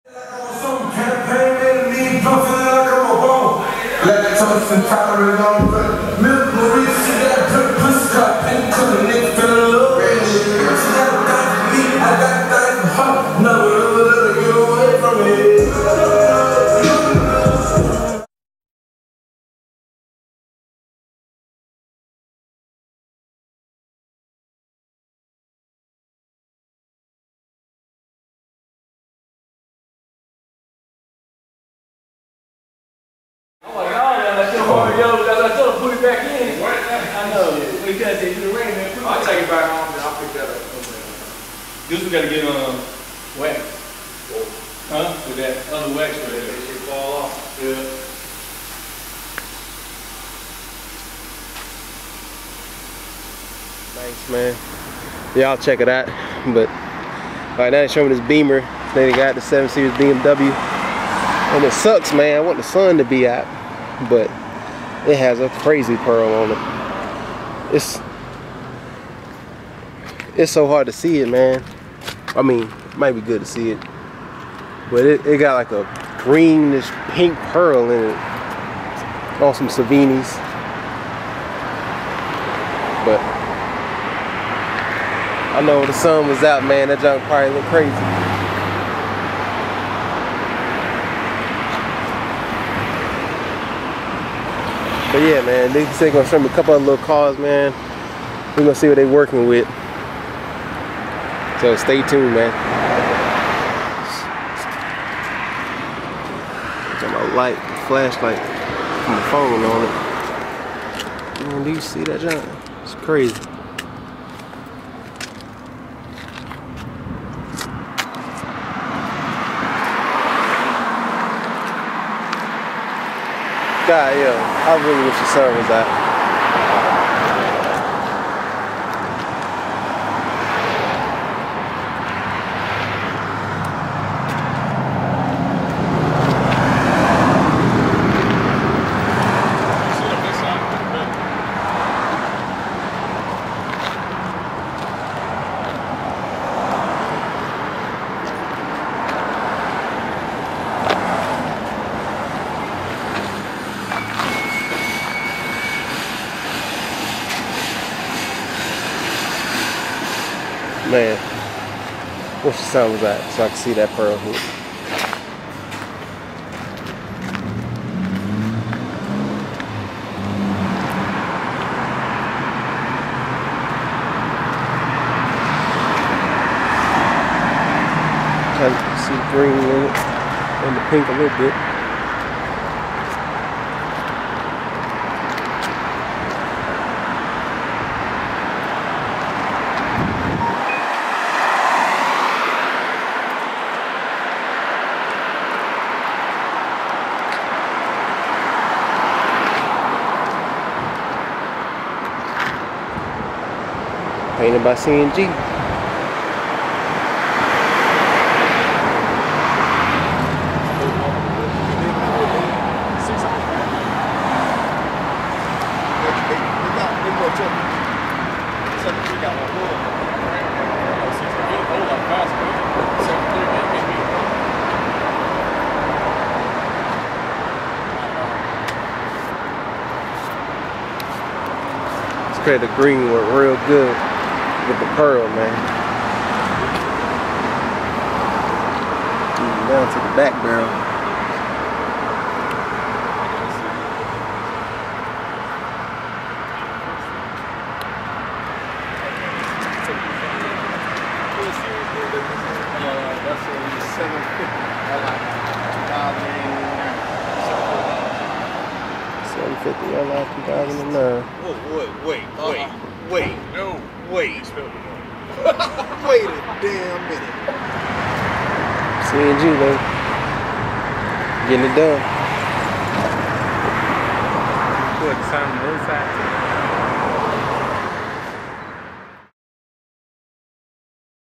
Green/Pink/Blue Pearl White BMW 750Li on 22" Savini Concave Wheels - 1080p HD
A 2009 BMW 7 Series on 22" Savini Forged SV37 XC wheels... With a HEAVY Pearl paint job! White BMW 750 Li with a green/pink/blue-ish pearl in the paint. Even the wheels change colors from green to blue!! Great paint job on this one.